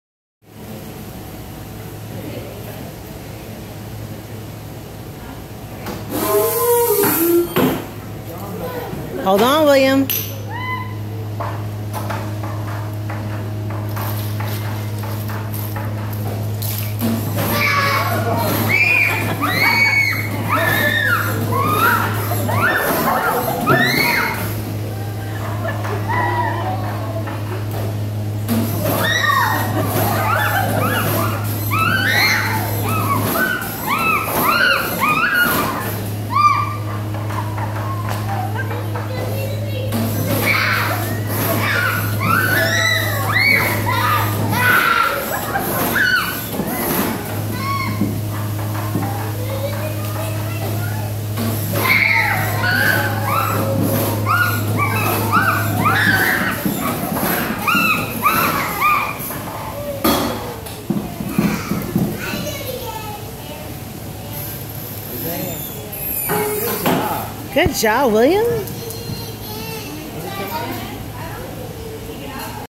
Will, the loudest screamer on the Rollercoaster
U.S. Space and Rocket Center, Huntsville, Alabama, July 4th, 2016